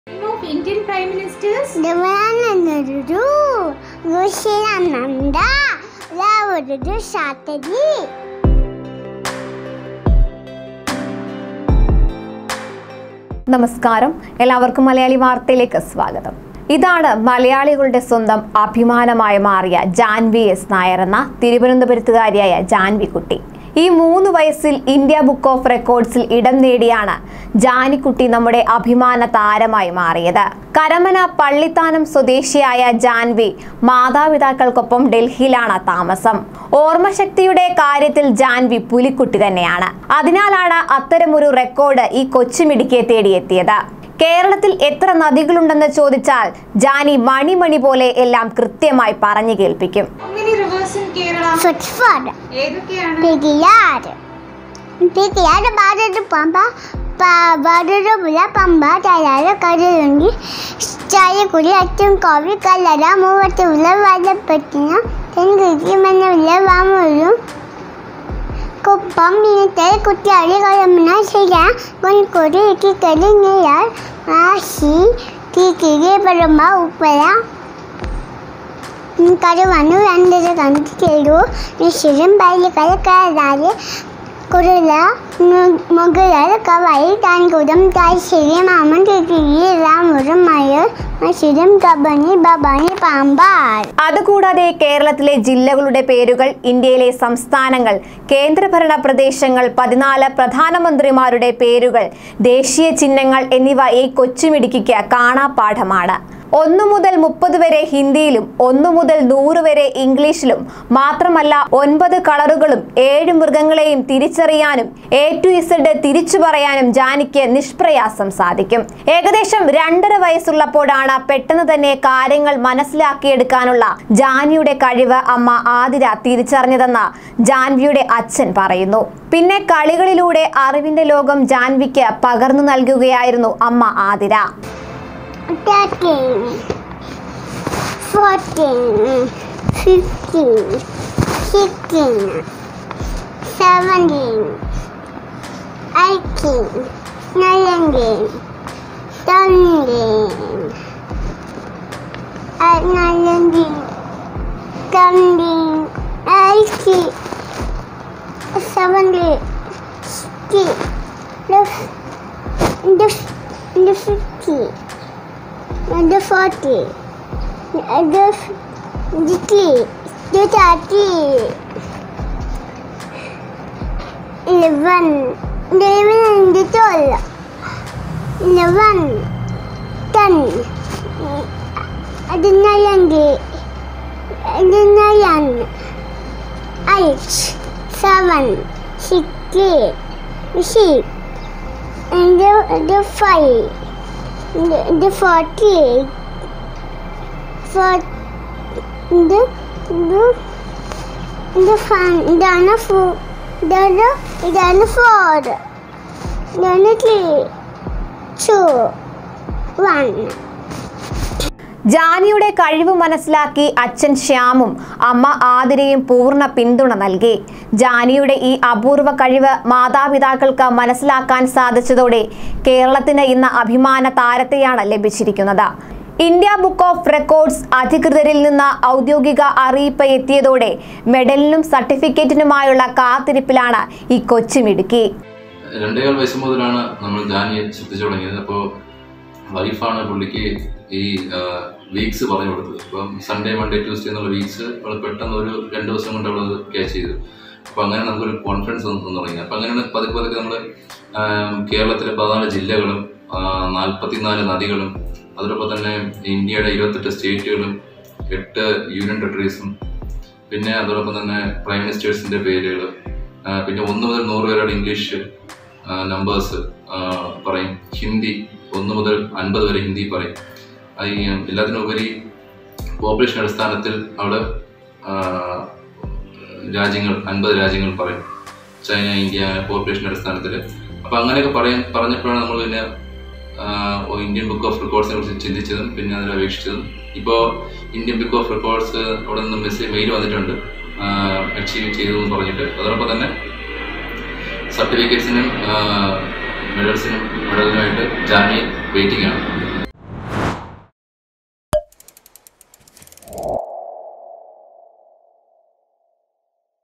India_book_of_records- ഈ കൊച്ചു മിടുക്കി
കേരളത്തിന്റെ
അഭിമാനമായി ജാൻവി
ഓർമ്മശക്തിയിൽ ഈ
കൊച്ചു മിടുക്കി
ഒരു അത്ഭുതം